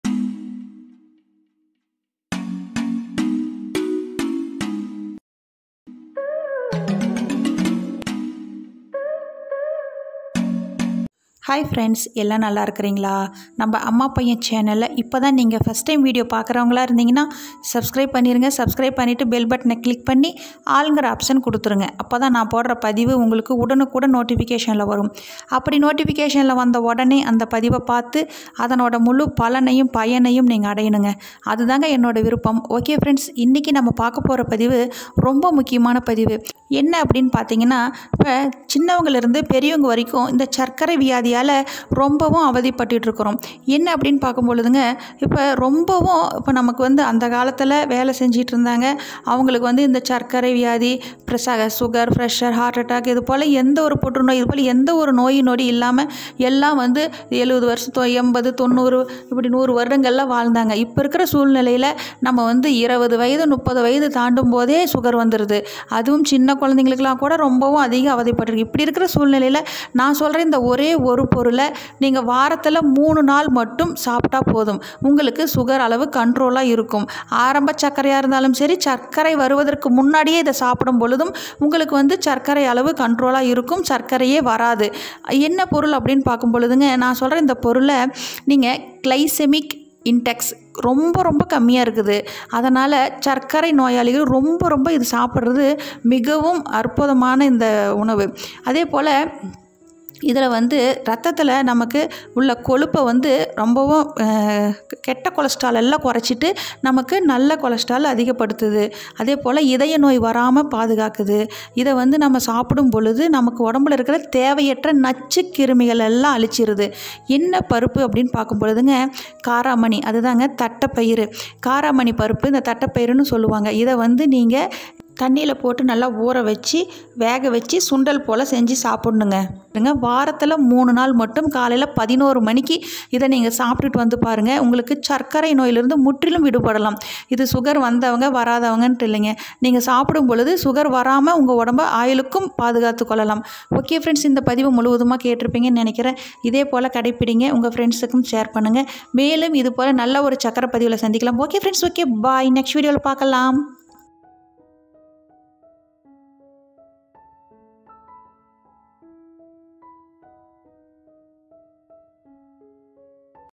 (3 நாள்) சர்க்கரையை குறைக்க சிறந்த வழி | சர்க்கரை நோயாளிகள் இனி கவலைப்பட வேண்டாம் |Diabetic patients
சர்க்கரையை குறைக்க சிறந்த வழி || சர்க்கரை நோயாளிகள் இனி கவலைப்பட வேண்டாம் || Diabetic patients வாரத்தில் மூன்று நாள் மட்டும் இதை சாப்பிட்டுப் பார்

Tamil, health, Tips, Vendhayam, Diabetics, சர்கரை நோய், வெந்தயத்தை,

Thanks for your valuable feedback

By : Amma paiyan

 நீங்களும் உங்கள் குடும்ப மருத்துவரை அணுகி உடல் பரிசோதனை செய்த பின்னரே மருத்துவரின் ஆலோசனைக்குப் பெற்ற பிறகு நீங்களும் பயன்படுத்த வேண்டும் ❎ இதை மீறினால் நிர்வாகம் பொறுப்பல்ல !! ❎